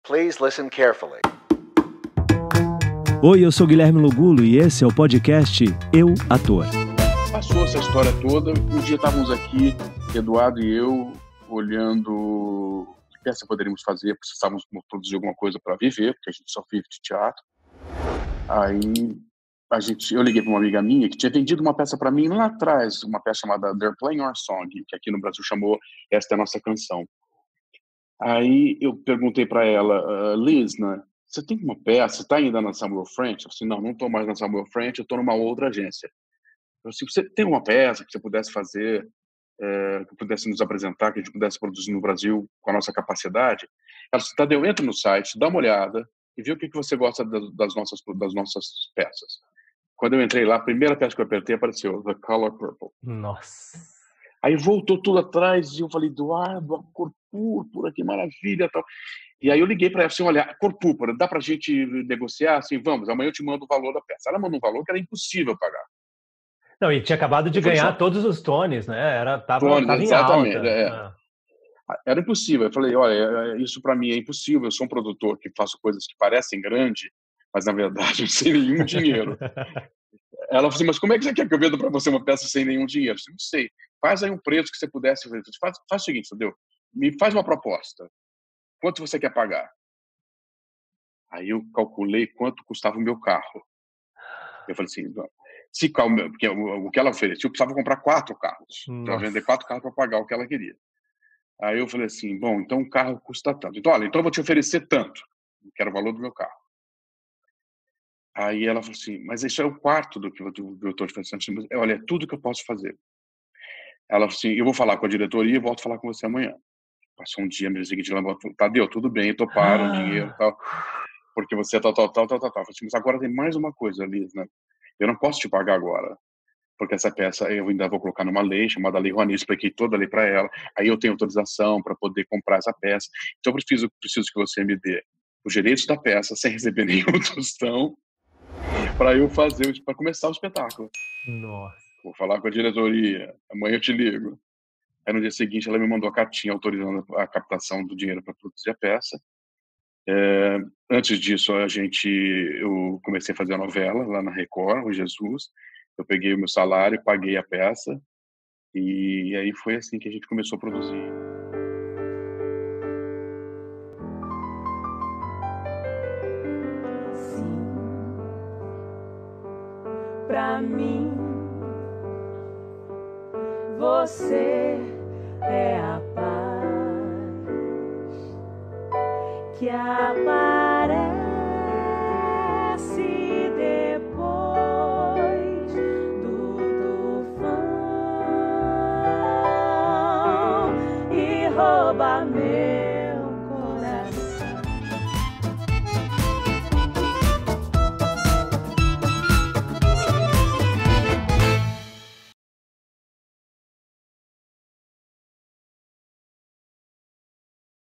Tadeu Aguiar | quanto vale o seu sonho?
Neste trecho da entrevista o ator e diretor Tadeu Aguiar fala sobre a compra dos direitos do musical da Broadway "A Cor Púrpura".

PODCAST disponível: SPOTIFY | APPLE PODCAST | GOOGLE PODCAST | DEEZER